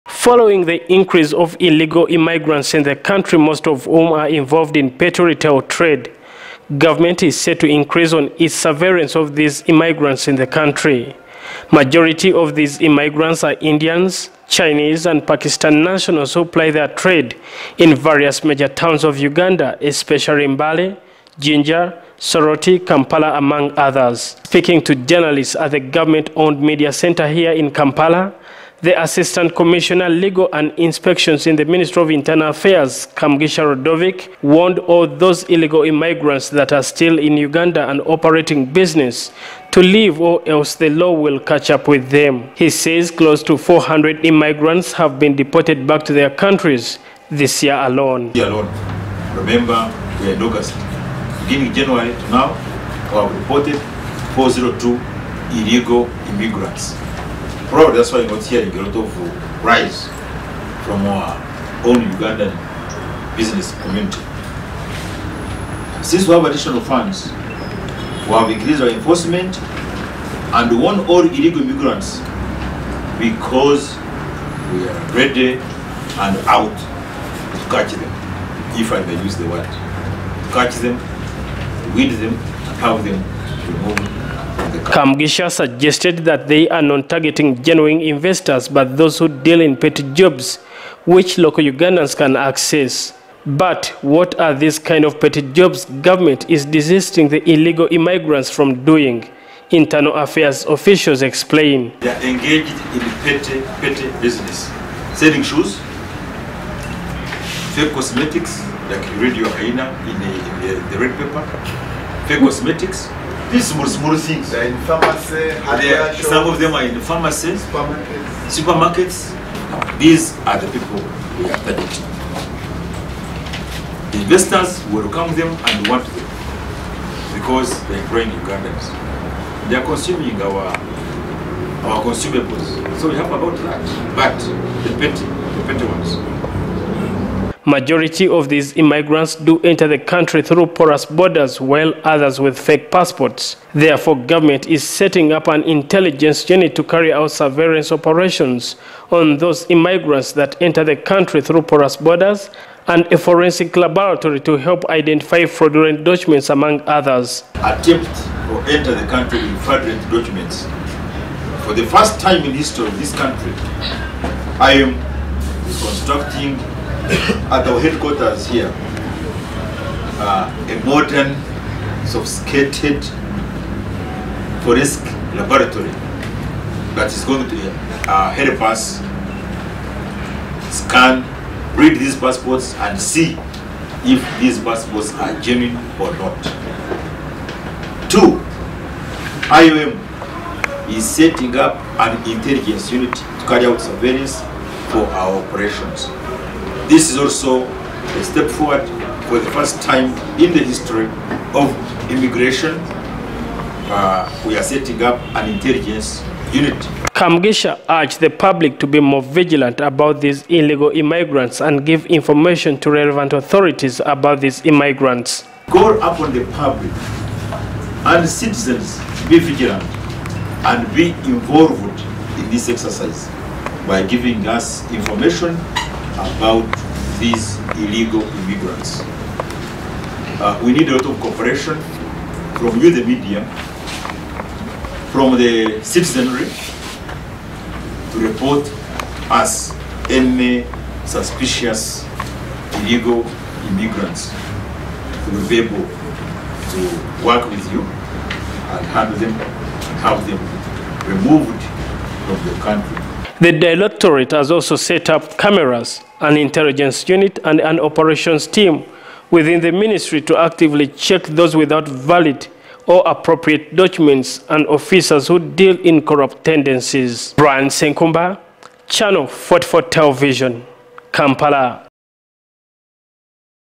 Ugandan Immigrants Plying Small Trades to be Deported
Foreign Immigrants plying trade in Supermarket, Shops and Clothes have been warned to stop or face Deportation and Immediate arrest.